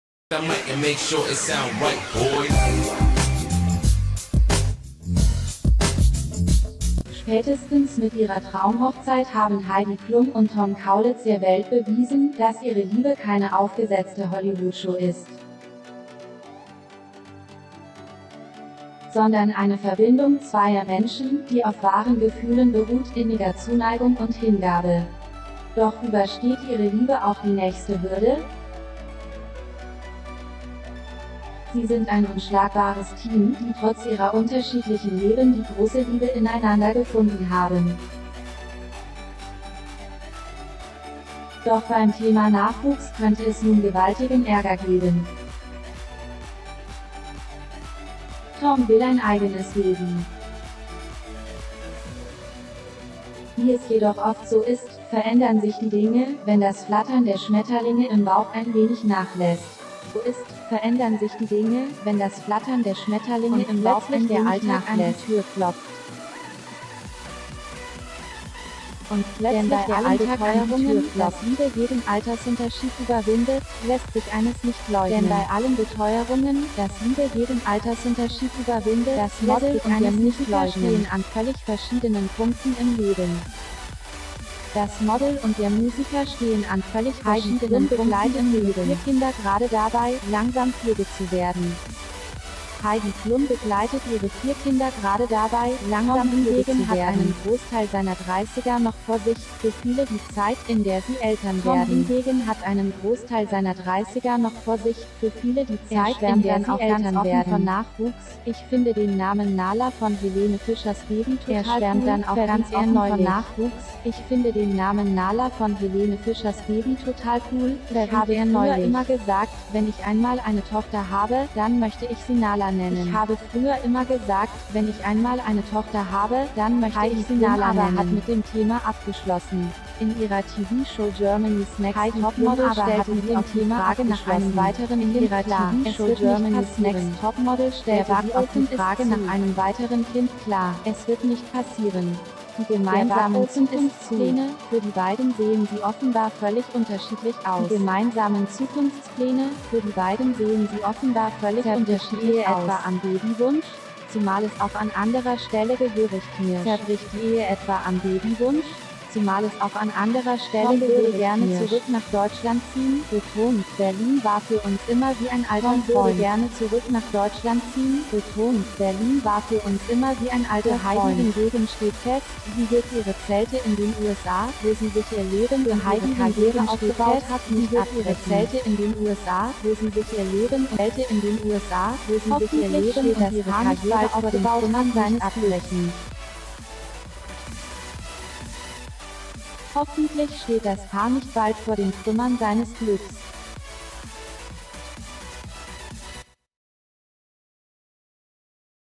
Heidi Klum & Tom Kaulitz: Traurige Babynachrichten! Ihr Glück ist zerbrochen! (27/02/2022)
Heidi Klum & Tom Kaulitz: Traurige Babynachrichten! Ihr Glück ist zerbrochen!
Eventfirma verriet: Wie Tochter Helene Fischer bei ihrer Hochzeit auftrat